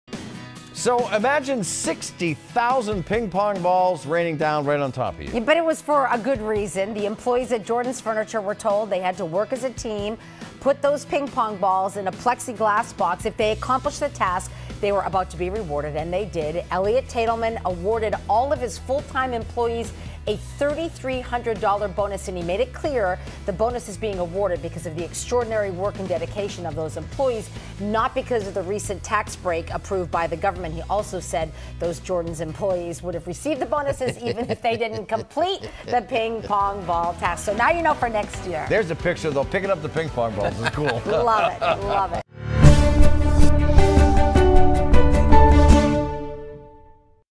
5 For Good: Popular furniture store gives bonus to workers in funny manner
Imagine 60,000 ping pong balls raining down on top of you... but for a good reason.